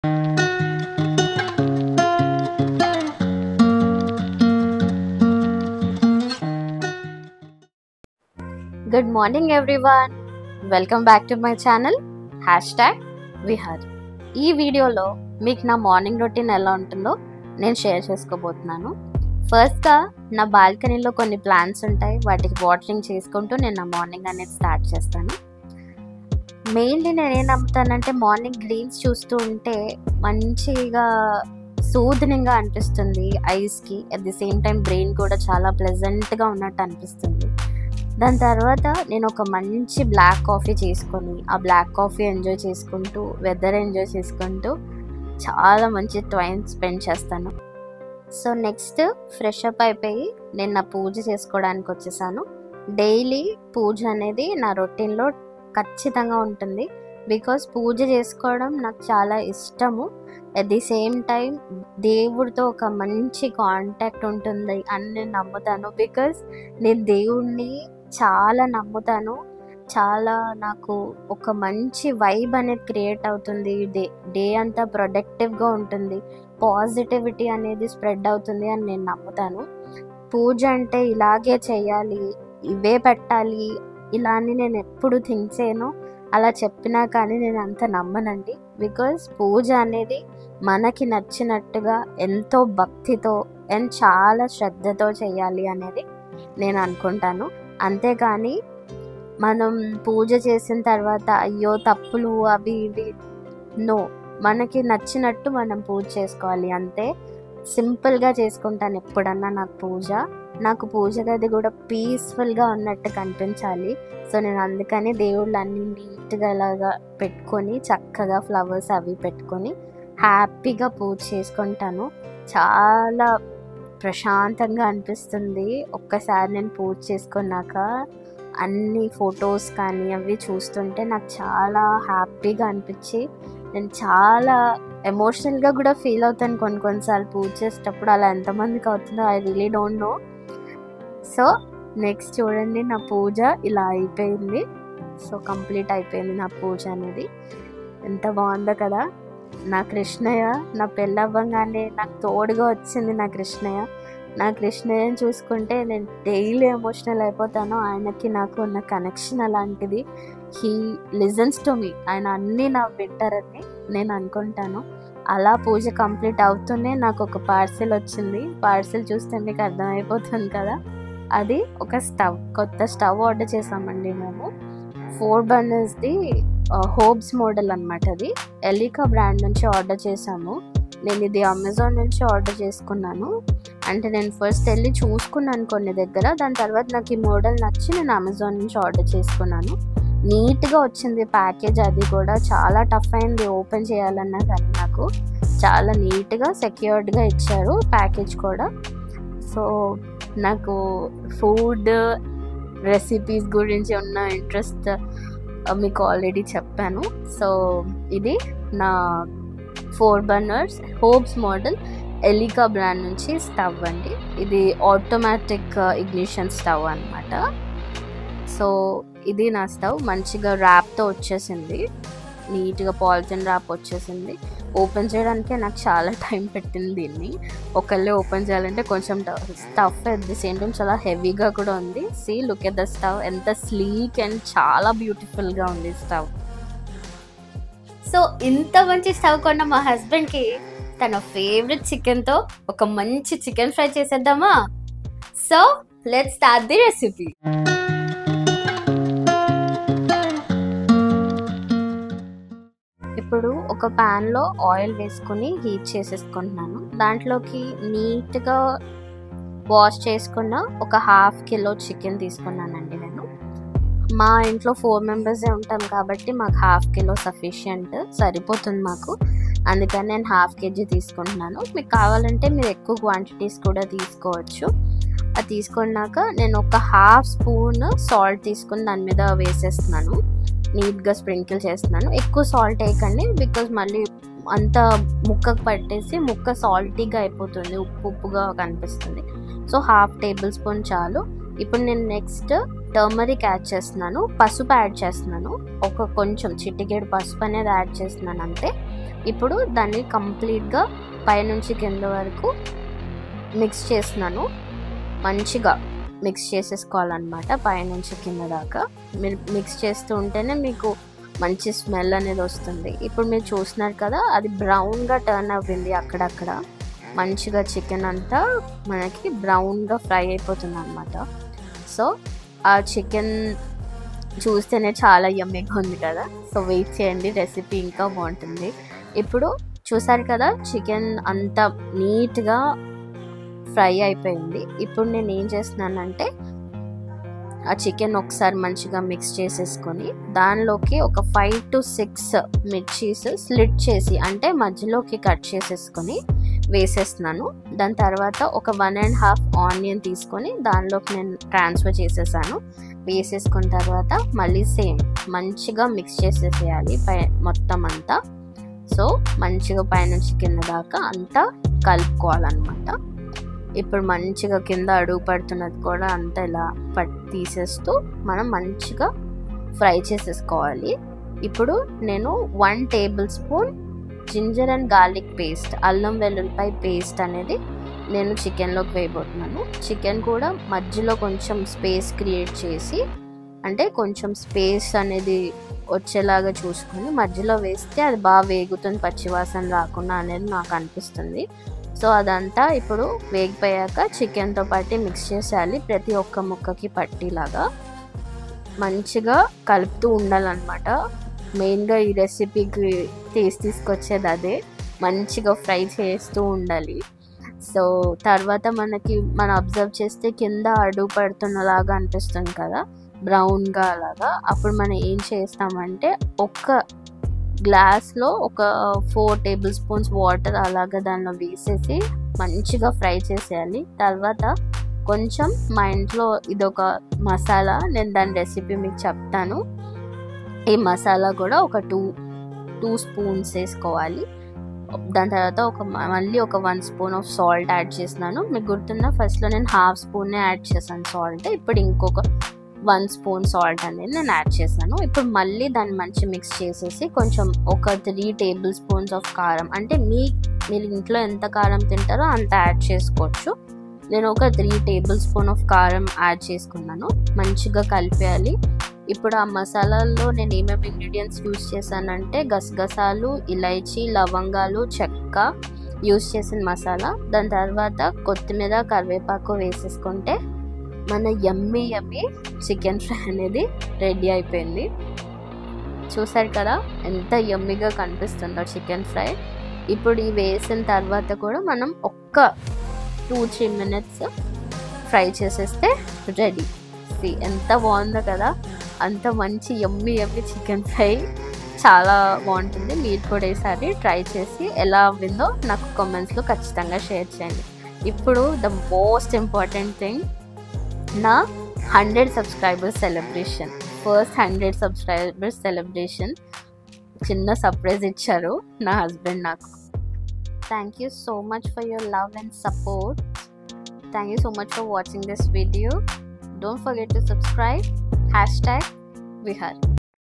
Morning Routine | My New Elica Stove | Special Chicken Fry | My First 100 Subscribers Celebration
In this video am telling you about my morning routine and few important moments about reaching first 100 subscribers. Bought a new hobes model  gas stove.